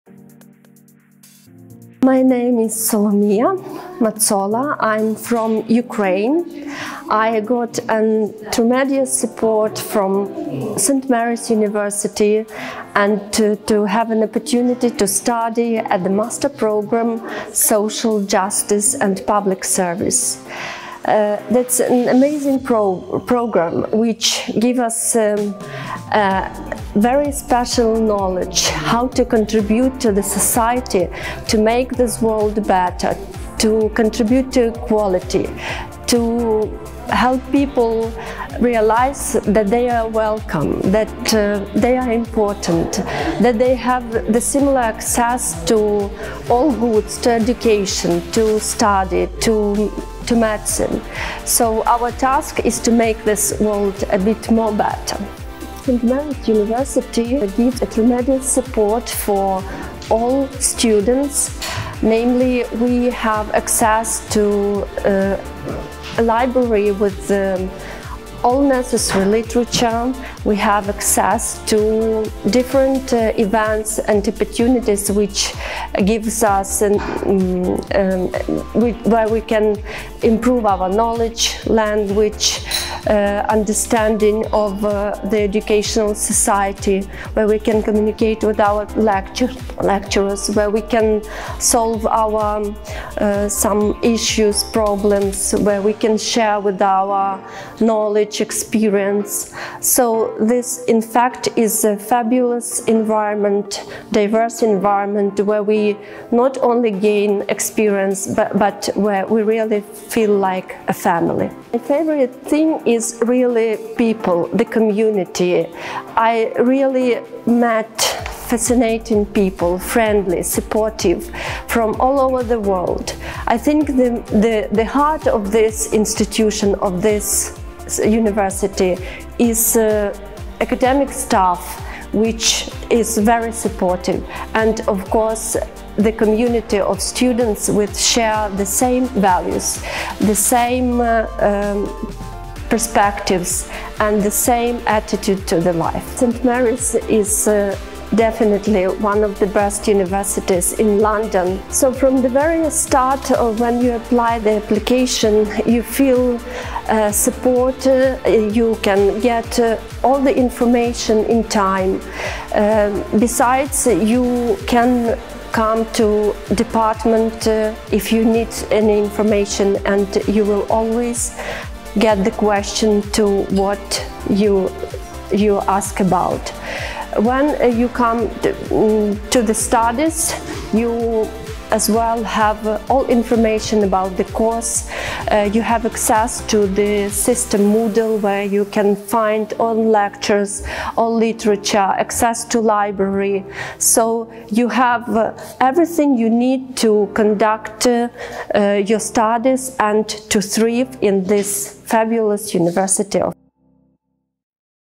MA Social Justice and Public Service Scholarship Student Experience
International Scholar Somlomia from Ukraine shares her experience studying MA Social Justice and Public Service at St Mary's